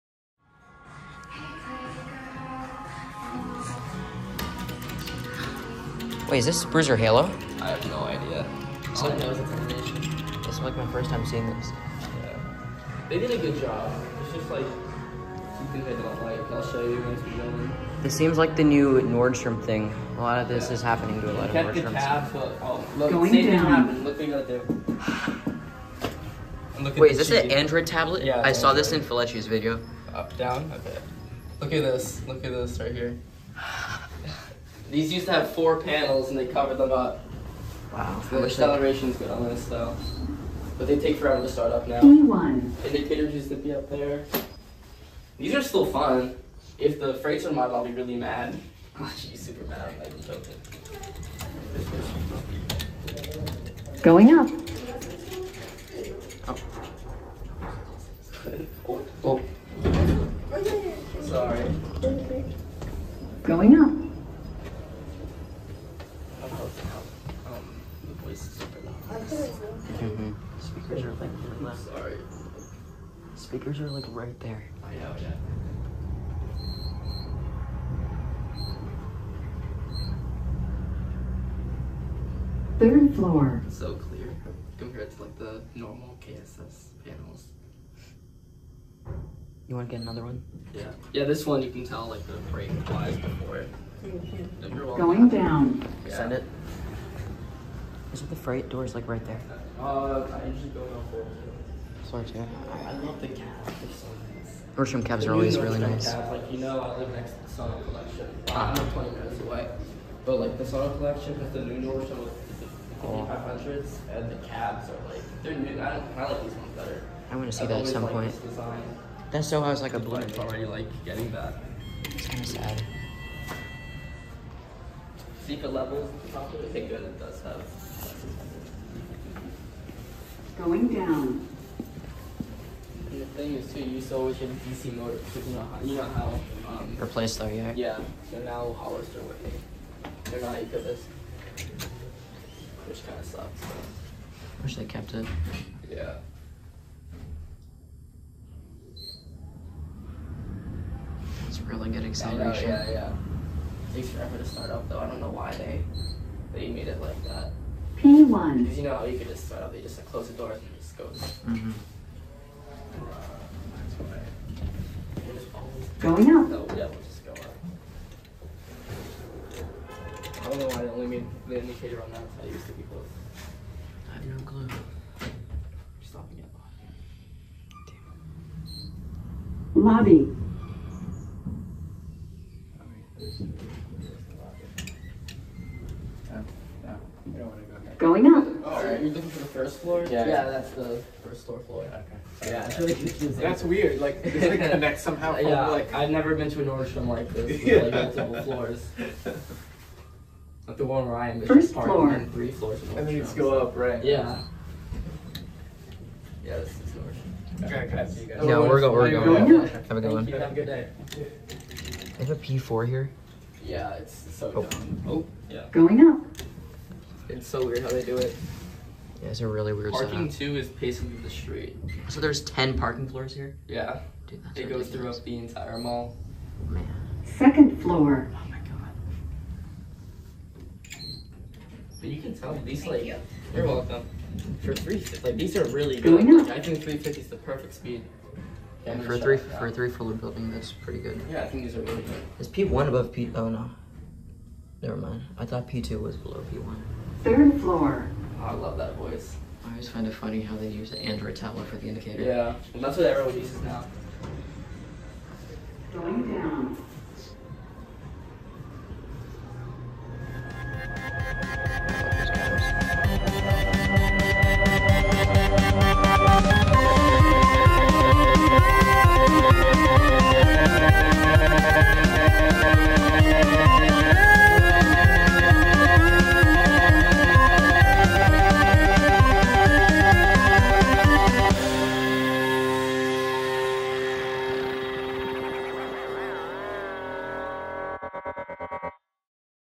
NICE KONE Traction Elevators - Nordstrom - The Westchester - White Plains, NY
(Recorded 1/14/24)

Some very nice, well behaving KONE mods. This seems like the new trend Nordstrom is leaning to. They did a great job with the mod. This is also my first time seeing these fixtures.

ft. @TechNerdNolan

Elevator Information:

Installation Details:
  Brand: Montgomery
  Mod Brand: KONE
  Installed: 1994
  Model: N/A
Fixtures:
  Hall Fixtures (Original): Innovation Universal
  Hall Fixtures (Mod): Innovation Bruiser Halo
  COP Fixtures (Original): Innovation Universal
  COP Fixtures (Mod): Innovation Bruiser Halo
Doors:
  Door Equipment: Wittur
  Door Configuration: Center Opening
Technological Specs:
  Speed: 350 FPM
  Capacity: 4000 lbs.
  Machine Type: Traction
  Set of: 3

Connect!